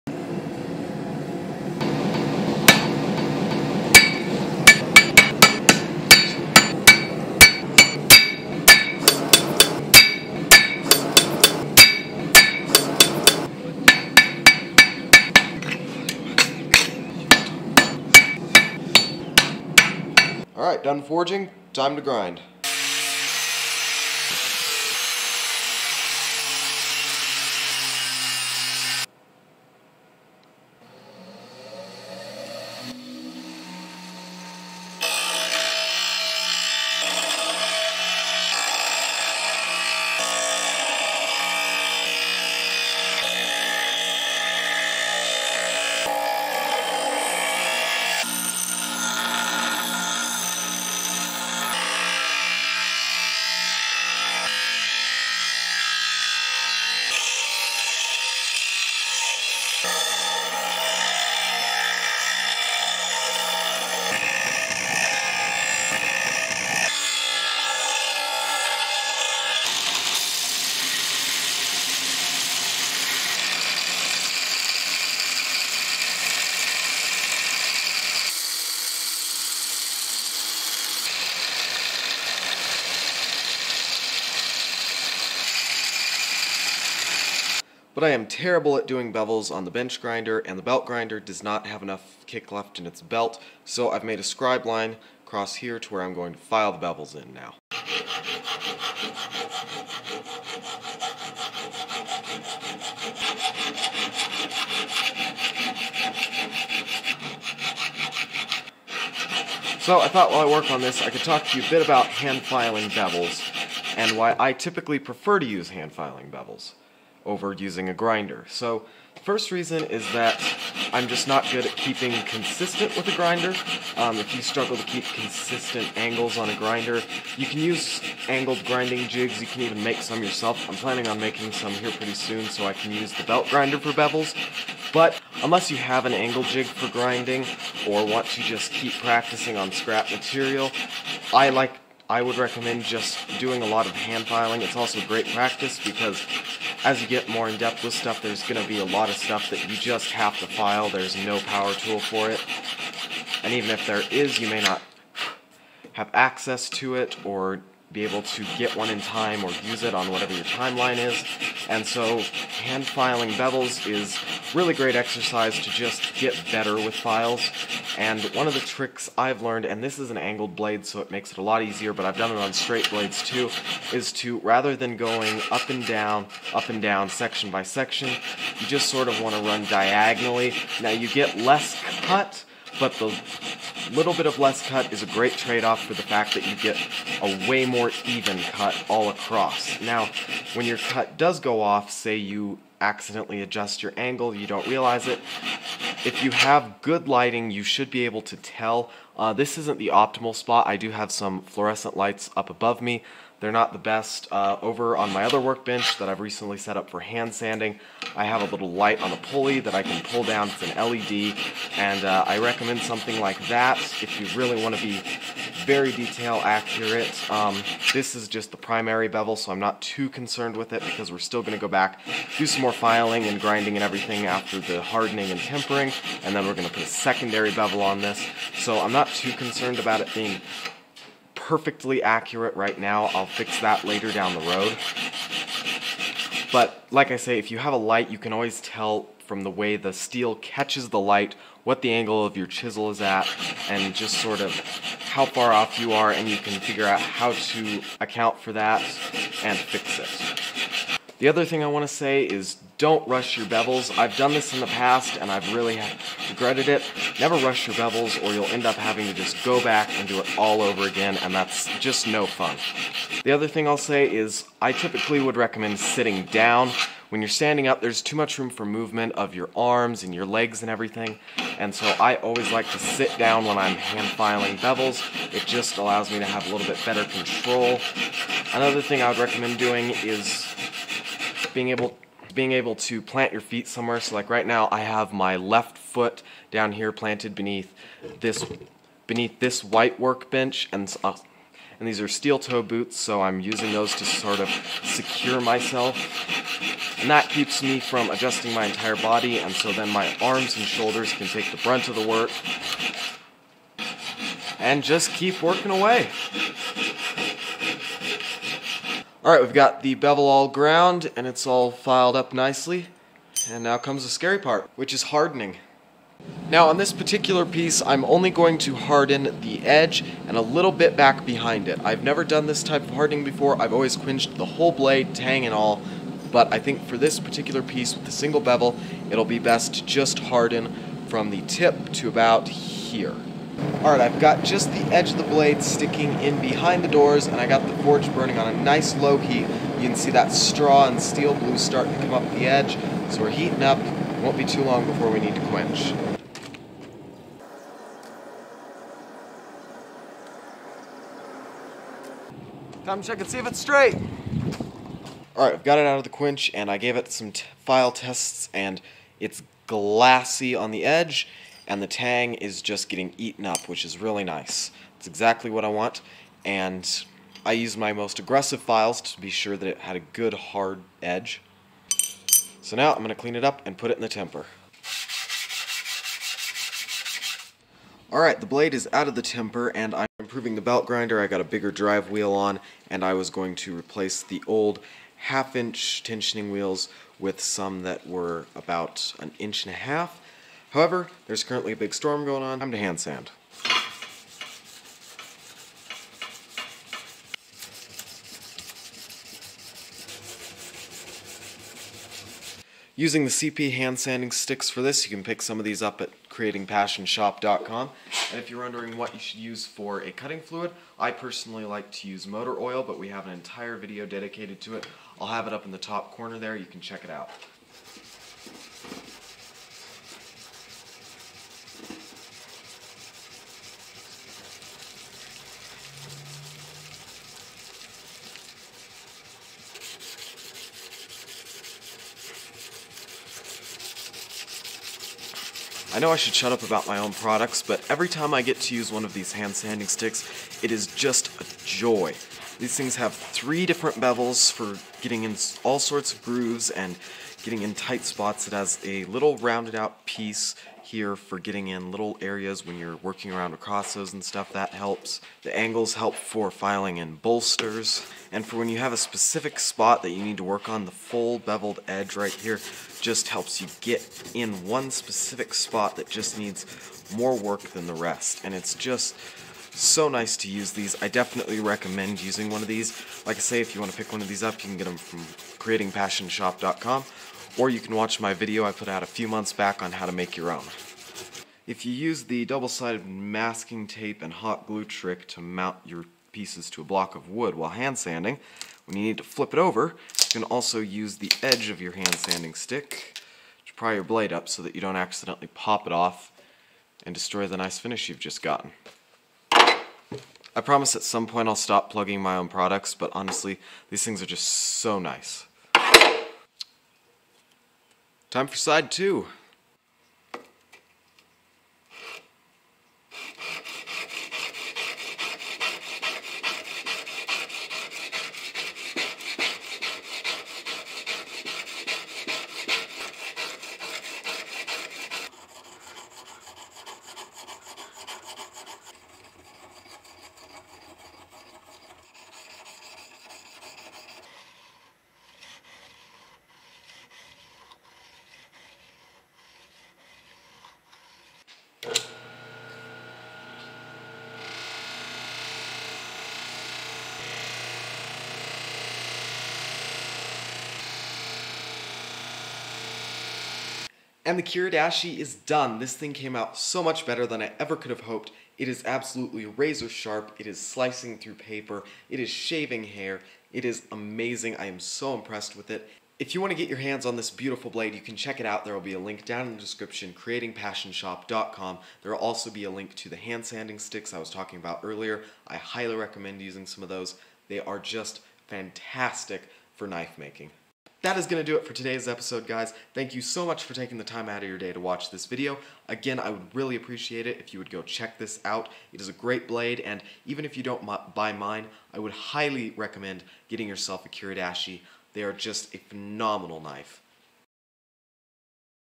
Making a Kiridashi! (marking knife)
In this week's episode I forge a kiridashi from 1080 high carbon steel! This blade has impressed me throughout all of its testing. I highly recommend getting a kiridashi.

Camera - KODAK PIXPRO AZ528